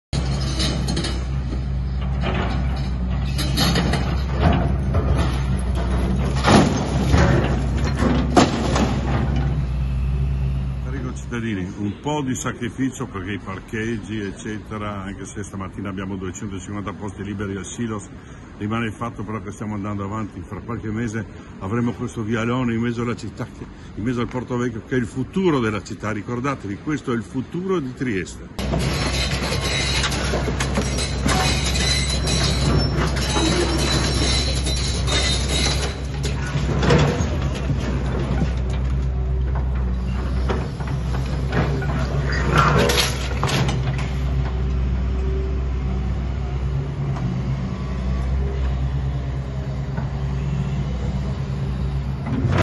Demolizione parcheggio Molo IV, Dipiazza: "Ricordiamo che questo è il futuro di Trieste"
Si entra nel vivo della demolizione del parcheggio del Molo IV a Trieste. Mercoledì mattina, 21 agosto, le ruspe hanno iniziato ad abbattere parte degli edifici. Quello slargo sarà infatti trasformato in una piazza verde completamente pedonale. Sul posto anche il sindaco di Trieste, Roberto Dipiazza: "Ai cittadini chiedo di fare un po' di sacrificio con i parcheggi, anche se al Silos al momento ci sono 250 posti liberi. Qui, tra qualche mese, avremo questo vialone al centro della città, un vialone verde. Questo è il futuro della città, il futuro di Trieste". Video Silvano